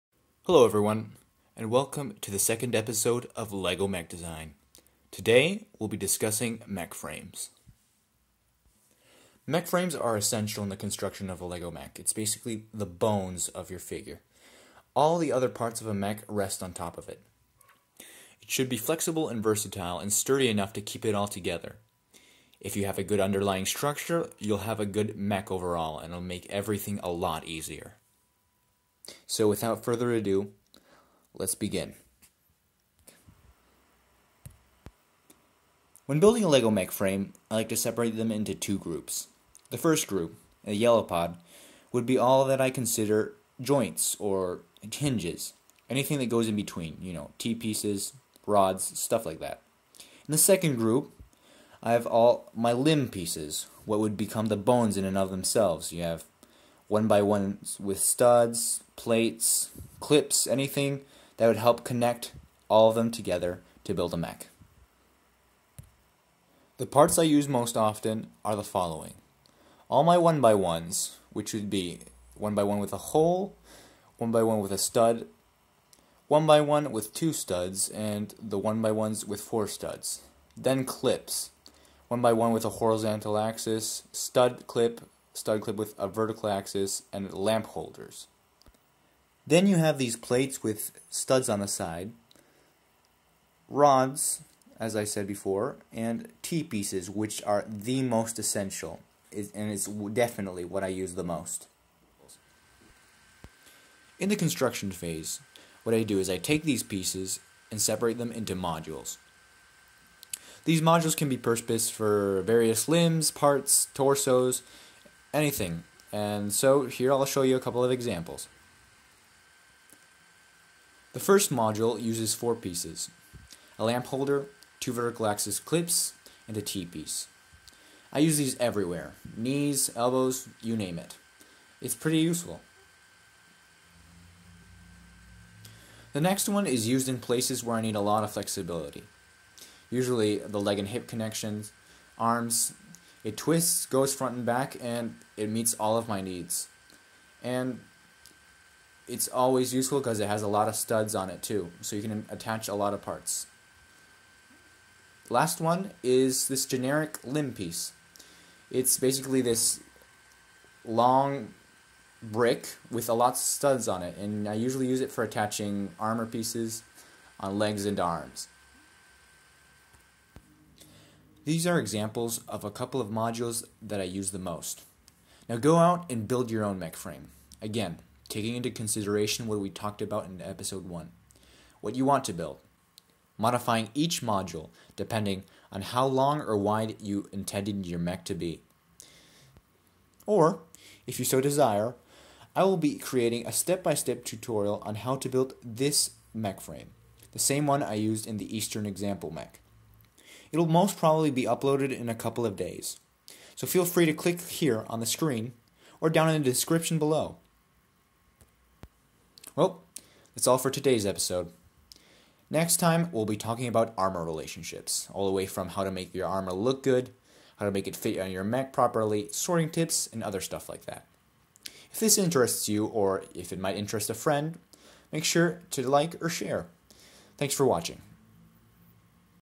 Lego Mech Design [02]: Mech Frames
In this episode we'll be covering the basics of structure in a Lego mech: the Frame.